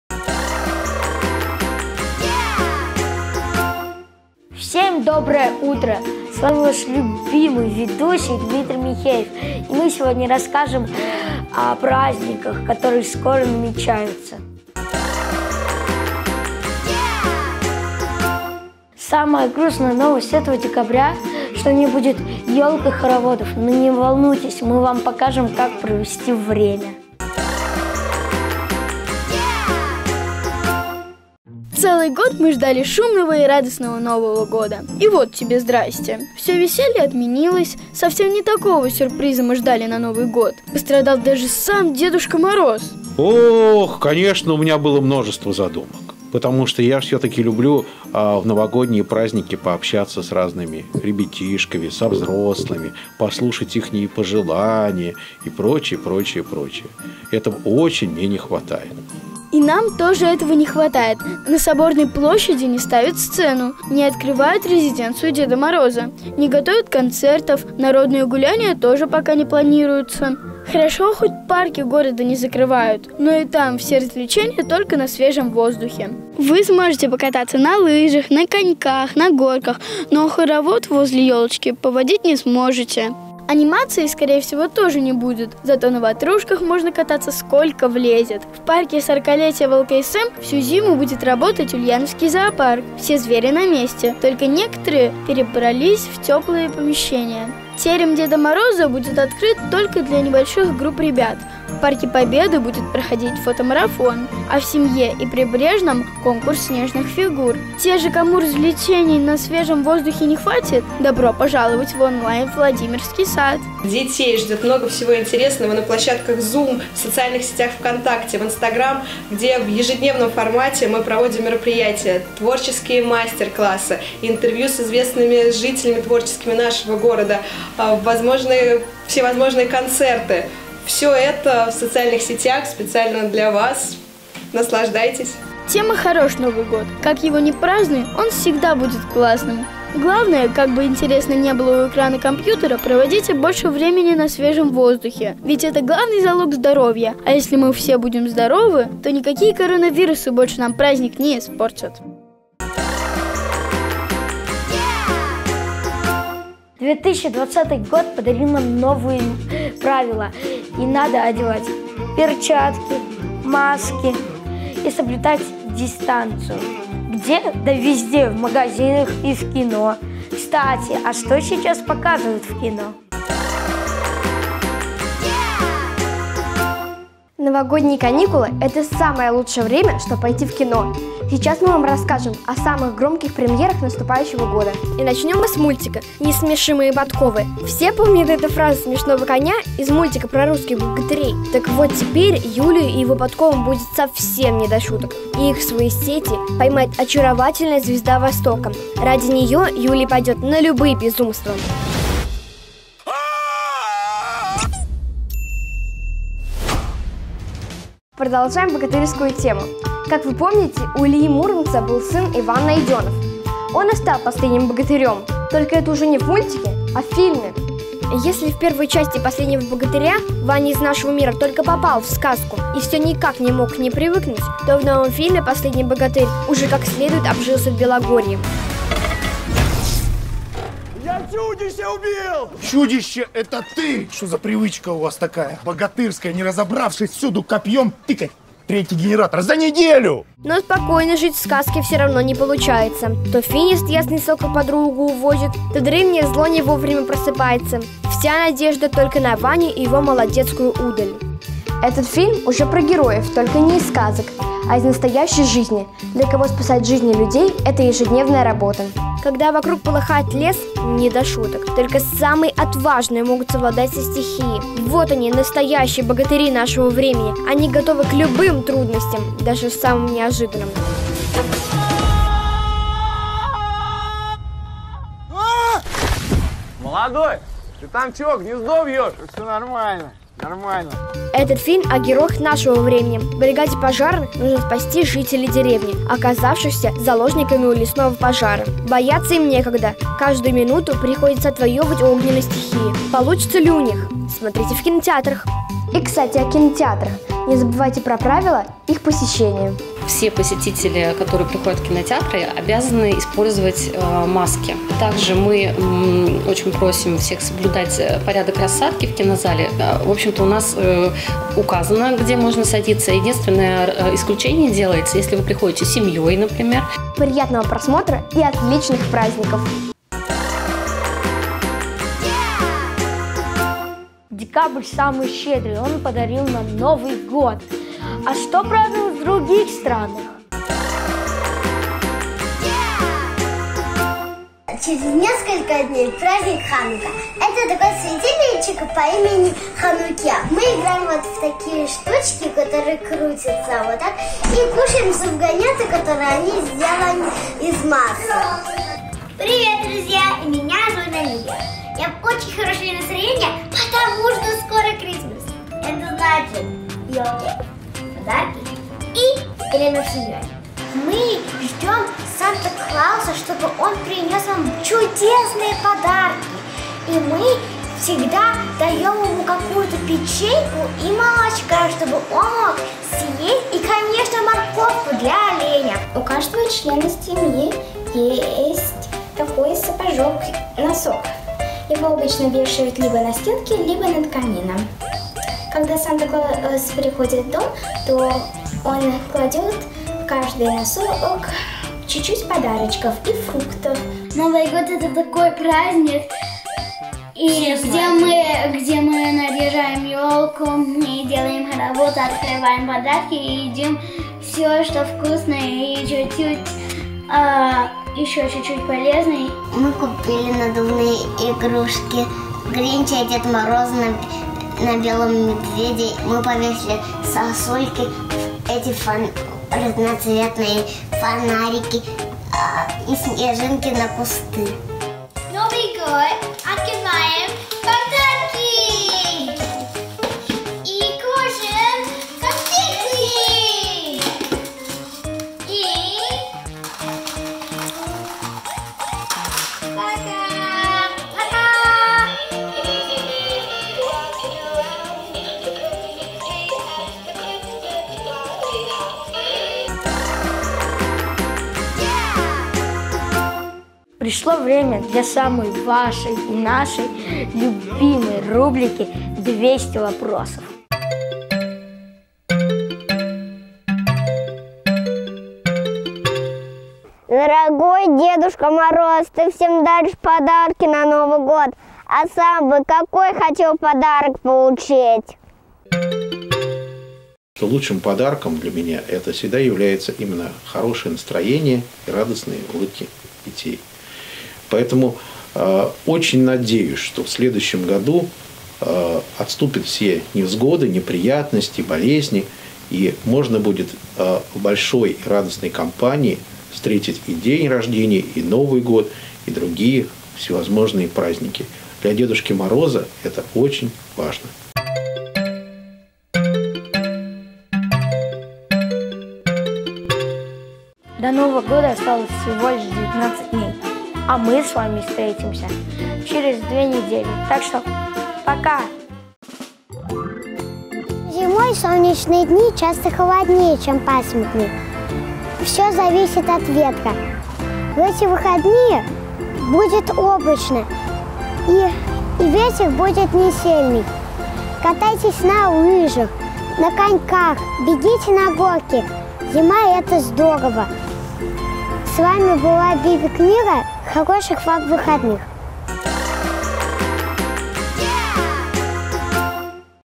ДВести: как Дед Мороз остался без Нового года и зачем на Рождество запасаться морковкой
До нового года осталось всего ничего! Пора уже строить планы на праздники. Кстати, их в декабре гораздо больше, чем вам кажется!